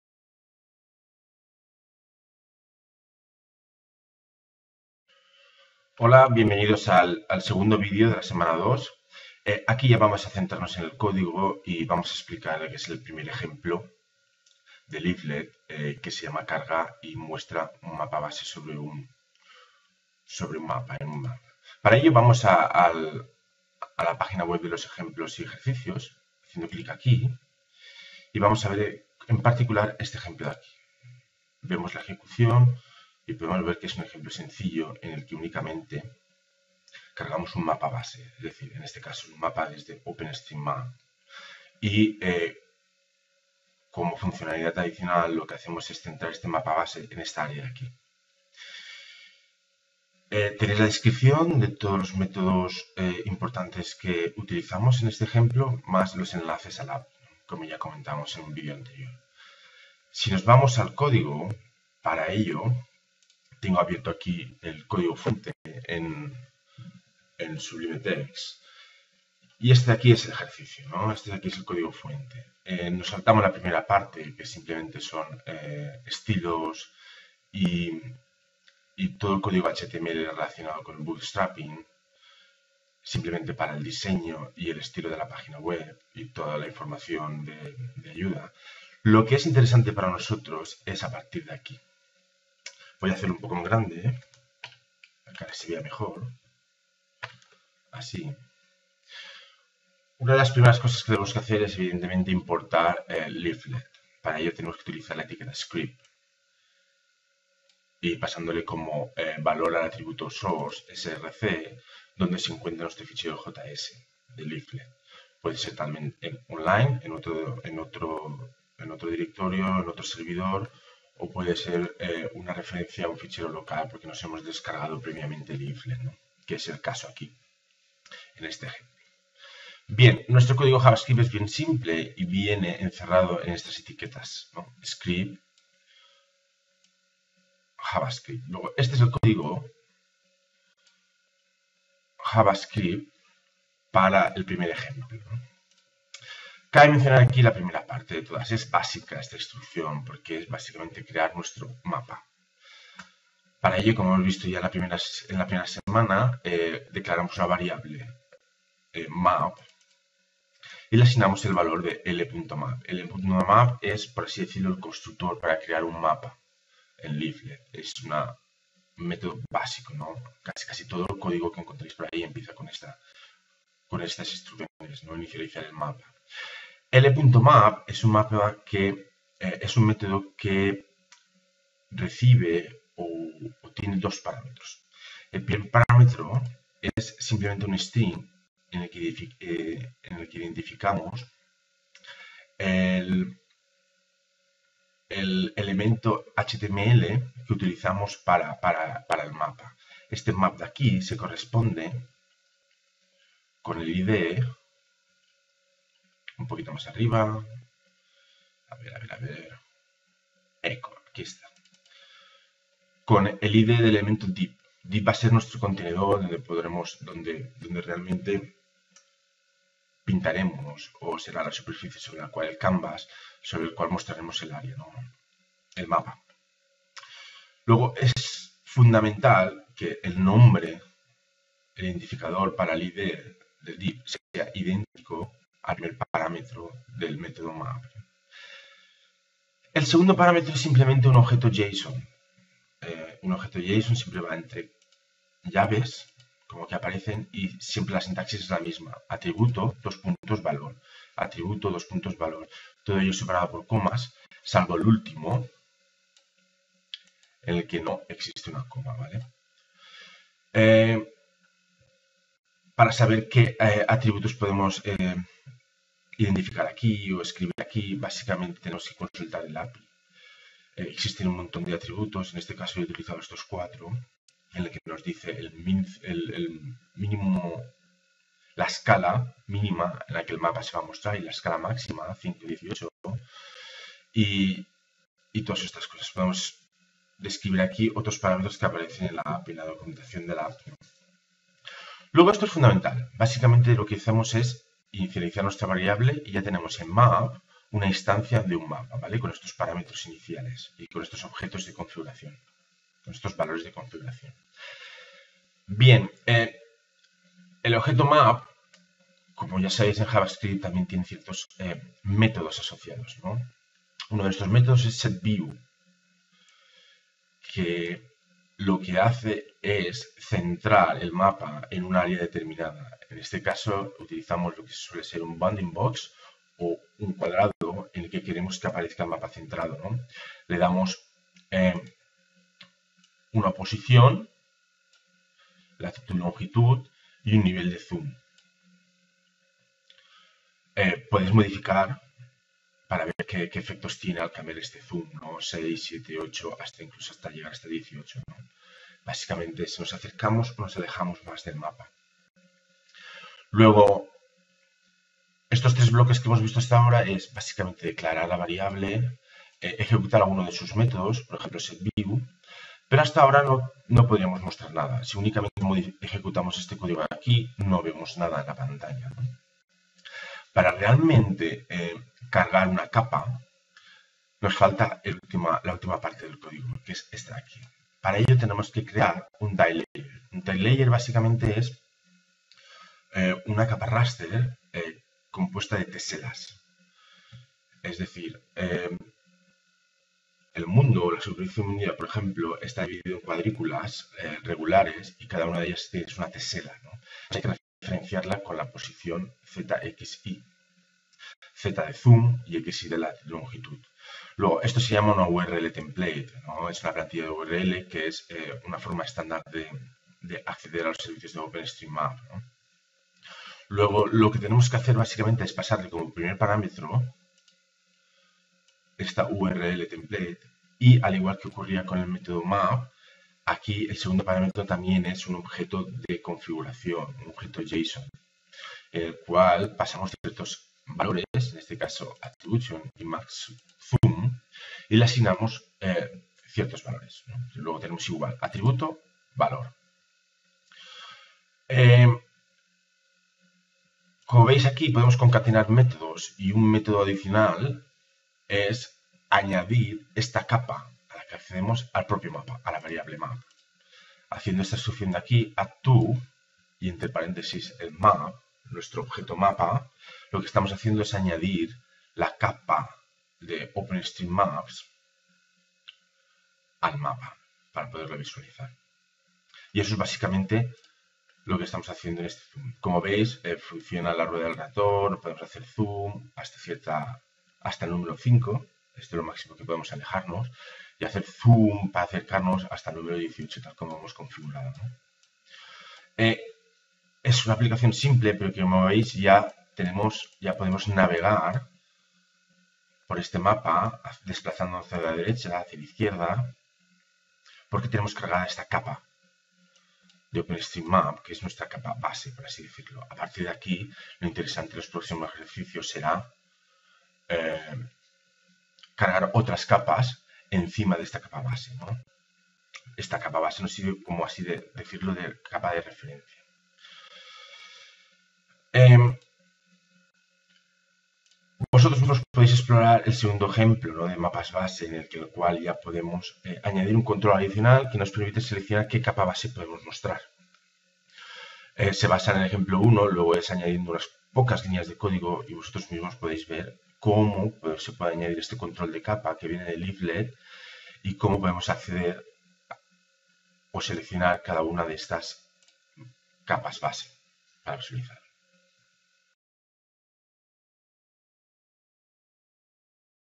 Cargar y mostrar un mapa base
Cómo seleccionar un mapa base con la librería Leaflet y visualizarlo